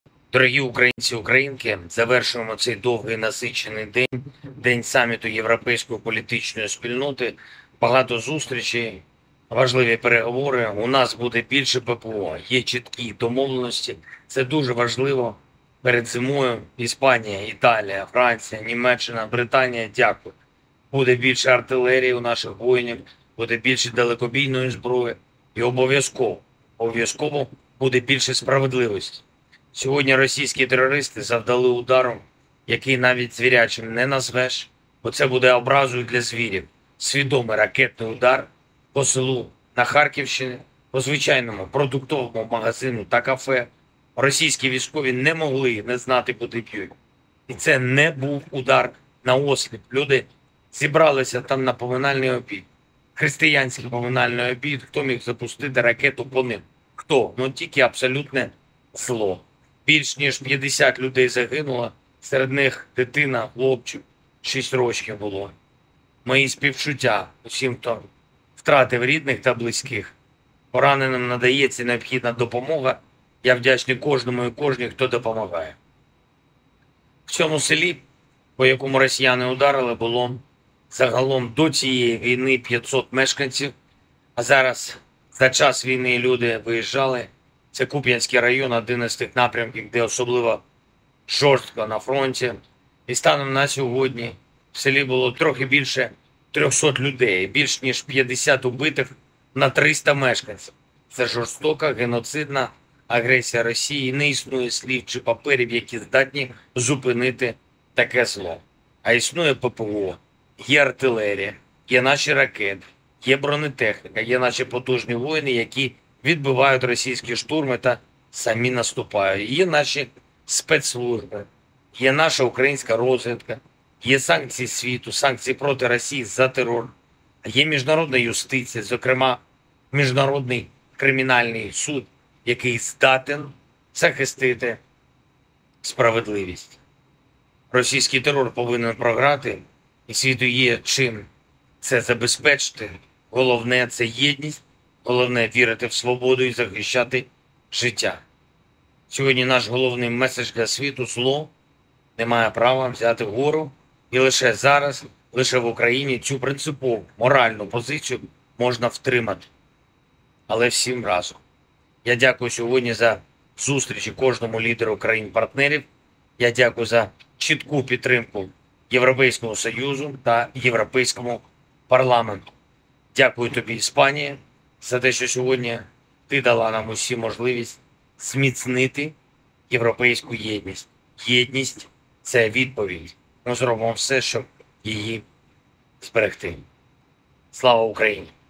Володимир Зеленський вечірнє звернення 5 жовтня 2023
Завершуємо цей довгий і насичений день. День саміту Європейської політичної спільноти. Багато зустрічей, важливі переговори.

У нас буде більше ППО – є чіткі домовленості. Це дуже важливо перед зимою. Іспанія, Італія, Франція, Німеччина, Британія – дякую!
Буде більше артилерії в наших воїнів. Буде більше далекобійної зброї. І обов’язково буде більше справедливості.

Сьогодні наш головний меседж для світу: зло не має права взяти гору. І лише зараз, лише в Україні цю принципову моральну позицію можна втримати. Але всім разом!

Я дякую сьогодні за зустрічі кожному лідеру країн-партнерів! Я дякую за чітку підтримку Європейському Союзу та Європейському парламенту.
Дякую тобі, Іспаніє, за те, що сьогодні ти дала нам усім можливість зміцнити європейську єдність.
Єдність – це відповідь.
Ми зробимо все, щоб її зберегти!
Слава Україні!

Джерело Телеграм Zelensky/Official